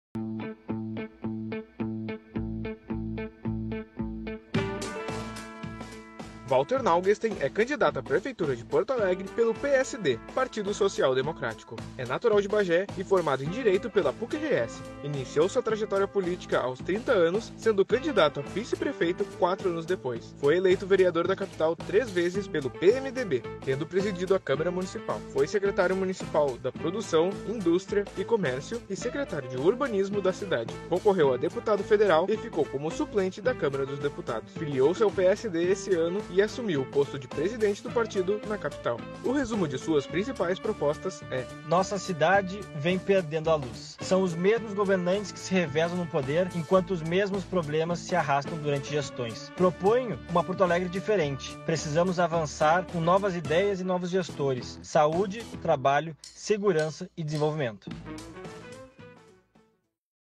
Quem São? - Valter Nagelstein
Vocês sabem "Quem são" os candidatos à prefeitura de Porto Alegre? Acompanhem nossa série de 13 episódios para conhecer cada um deles. Conheça agora Valter Nagelstein.